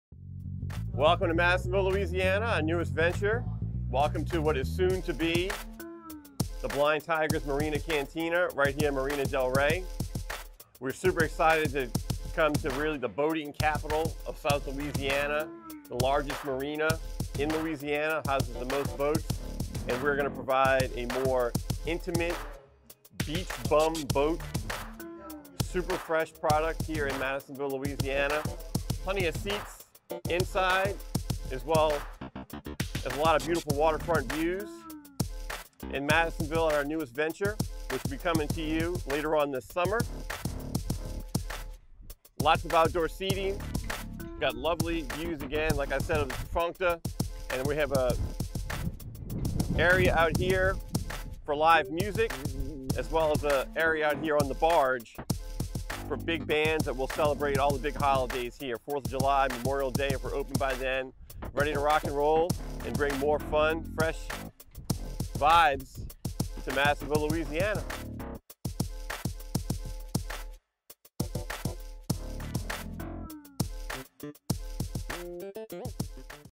TBT Marina Cantina | Madisonville, LA
An extension of The Blind Tiger, TBT Marina Cantina will be opening up soon in Madisonville, LA.